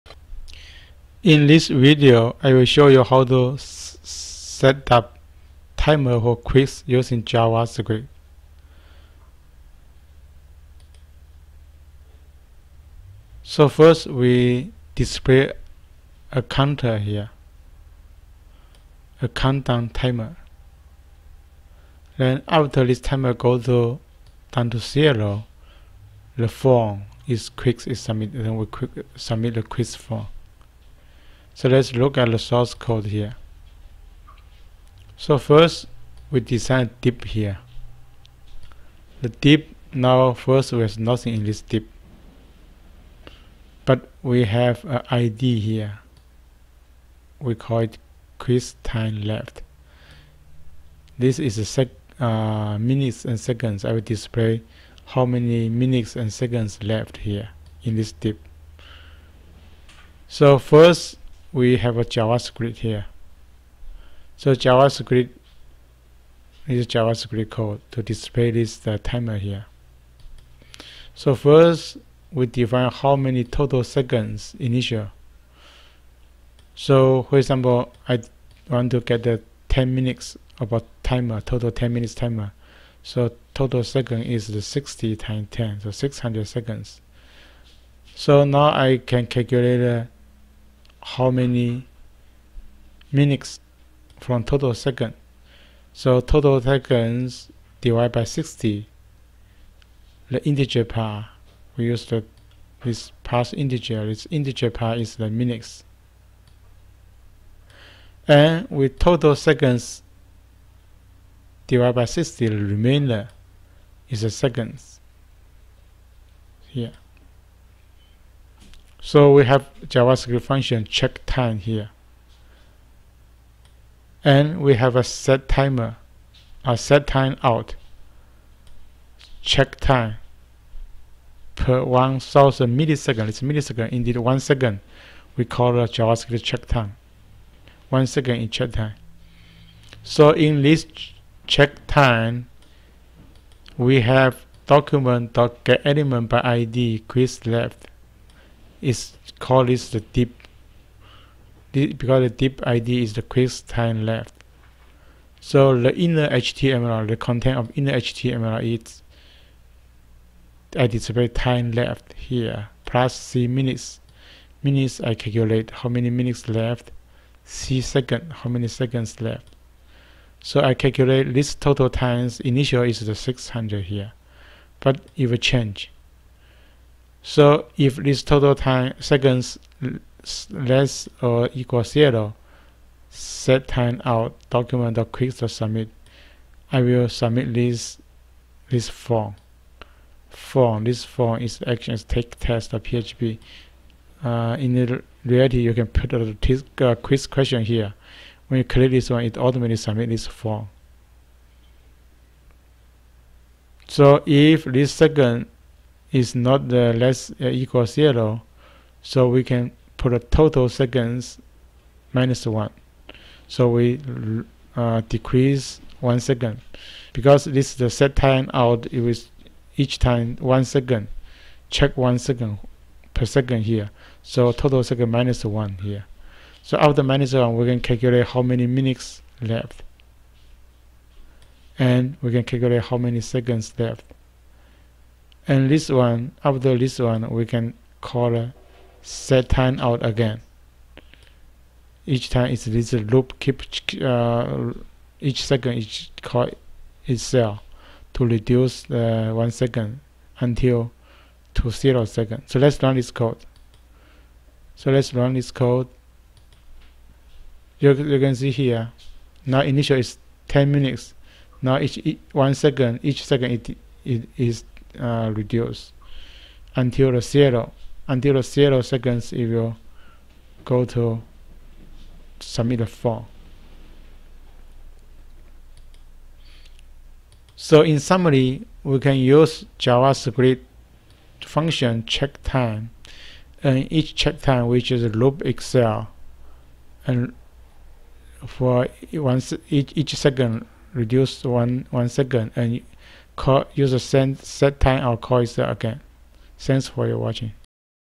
How to setup timer for Quiz using JavaScript
To setup timer for Quiz using JavaScript
1) To display a count down timer
2) When down to 0, the form quiz is submitted.
3) JavaScript  function setTimeout("CheckTime()",1000); is used for
function iteration approach.